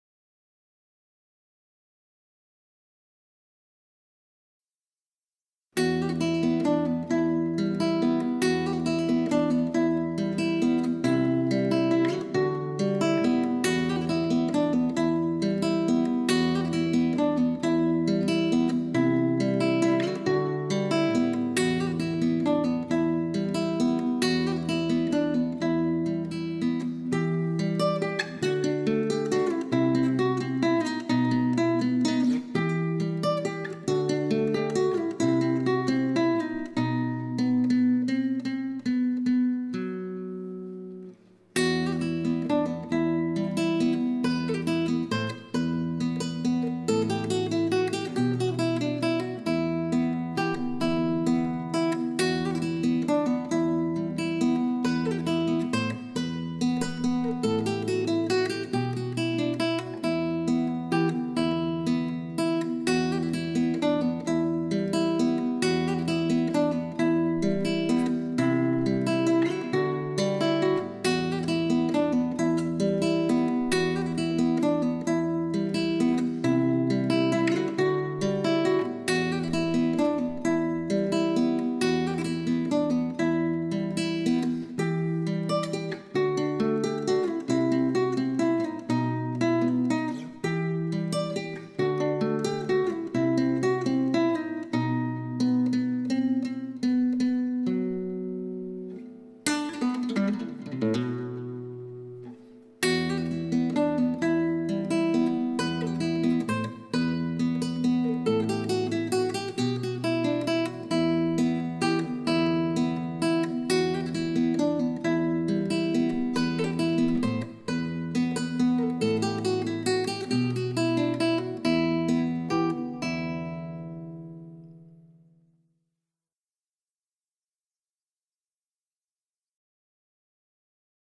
Original composition for Classical guitar.. Renaissance inspired..
My interest in Medieval and Renaissance music began many years ago and I was always looking to write something in this style for guitar.

Here is my interpretation of that music..

This piece was one of the first things I wrote and here is the first recorded version of it.

Recorded with a AKG C1000 into Cubase.